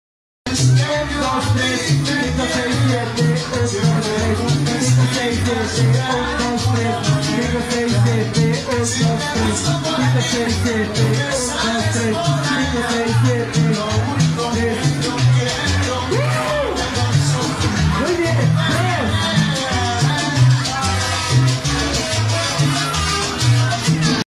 Salsa Club in Medellin
A group of us enjoying salsa dancing at a salsa club in Medellin.